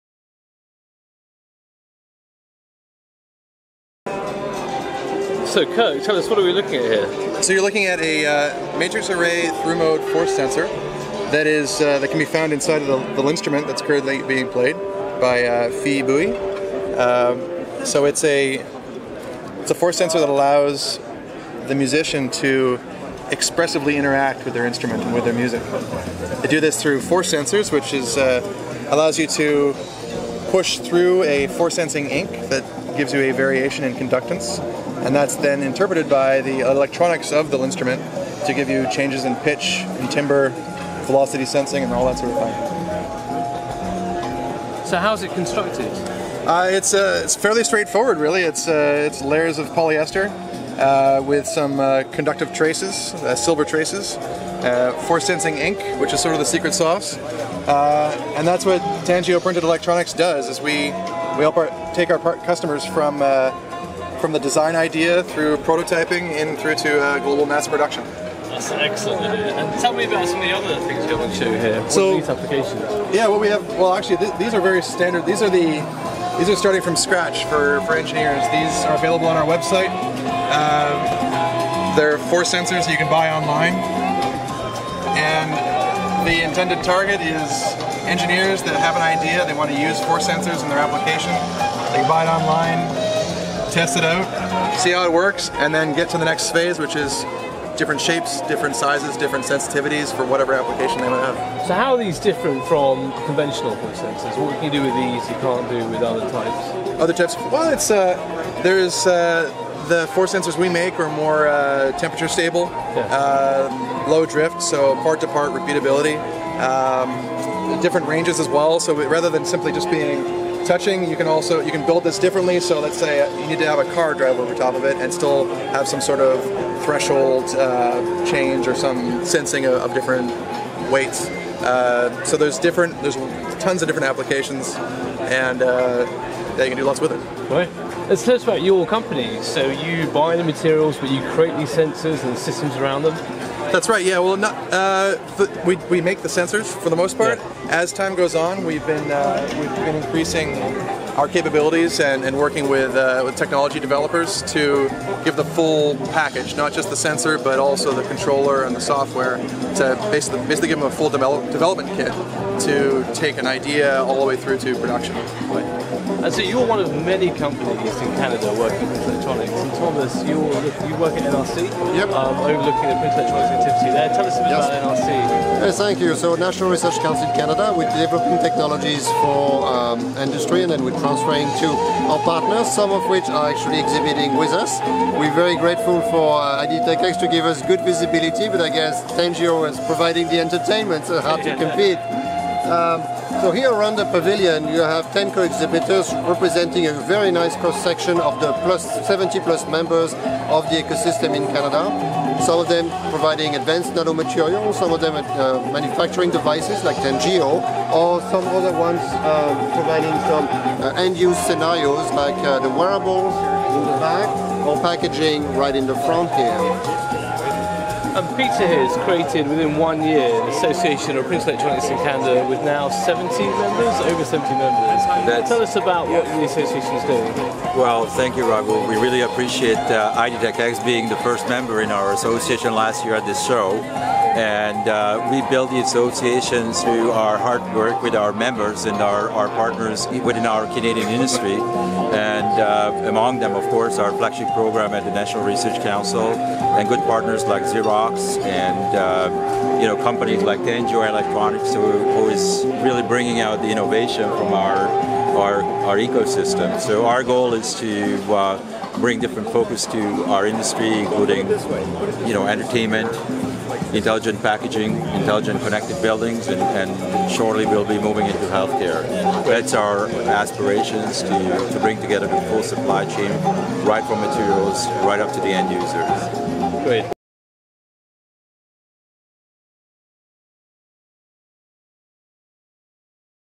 IDTechEx Interview: Tangio Printed Electronics and the National Research Council Canada
An interview with Tangio Printed Electronics and the National Research Council Canada at the Printed Electronics USA event.  Tangio Printed Electronics produce force touch sensors that are in advanced touch-sensing products and Tangio discuss the force sensor incorporated in the LinnStrument.  The National Research Council (NRC) is the Government of Canada's organization for research and development. NRC's Printable Electronics (PE) initiative coordinates key industrial areas - materials, ink, printing, and packaging - as a springboard for a profitable, large-scale PE sector.